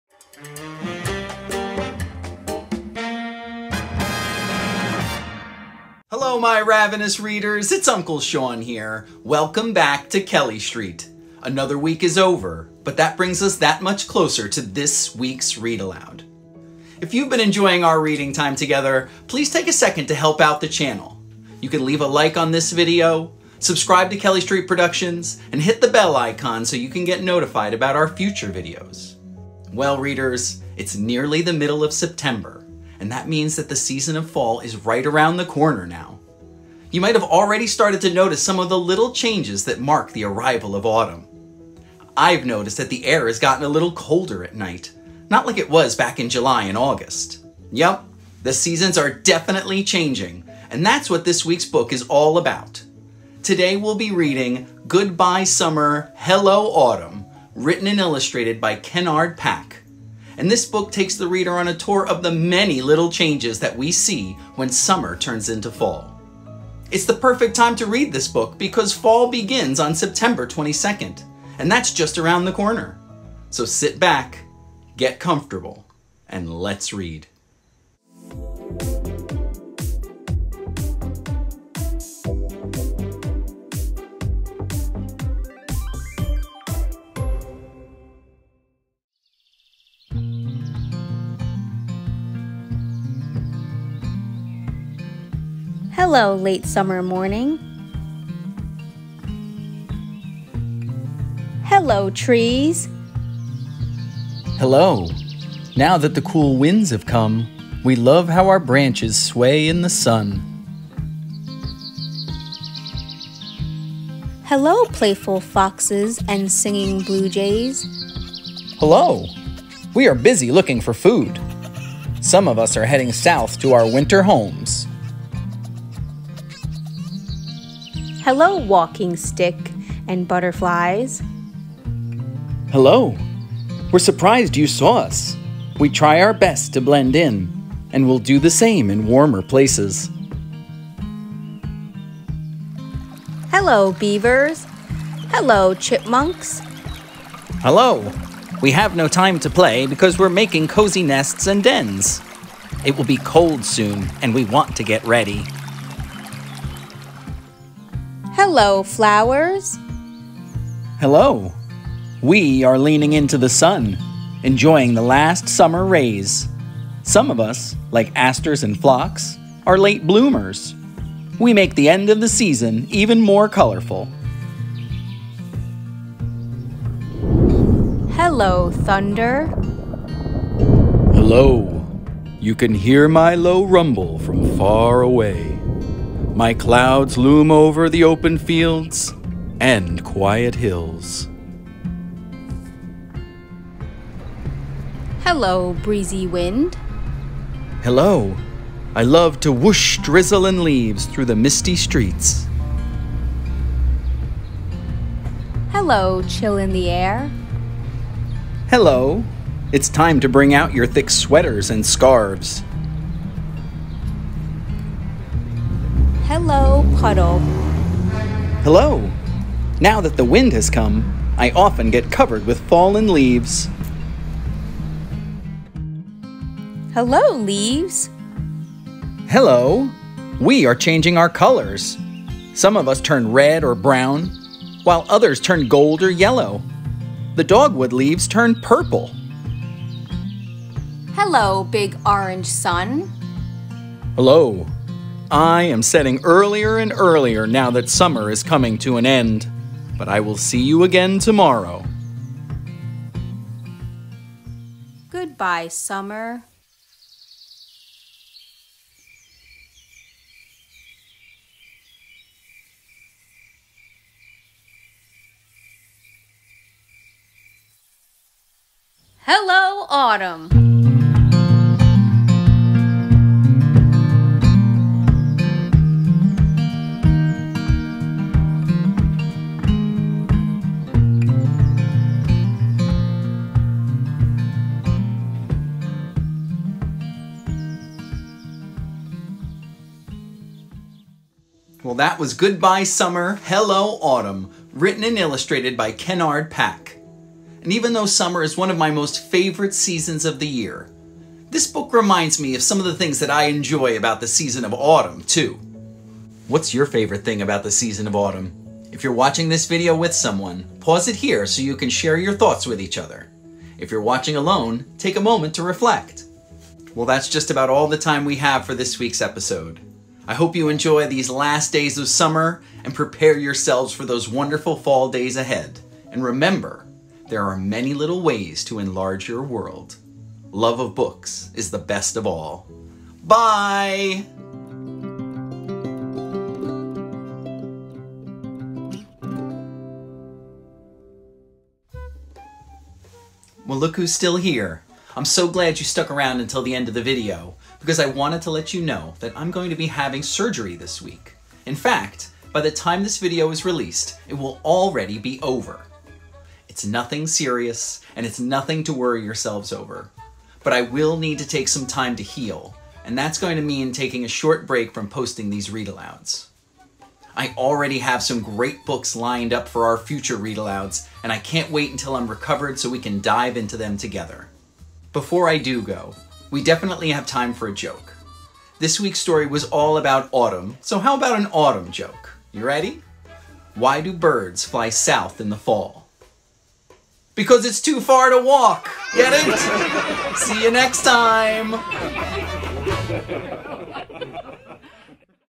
Read Aloud: Goodbye Summer, Hello Autumn
Read aloud of Goodbye Summer, Hello Autumn, written and illustrated by Kenard Pak.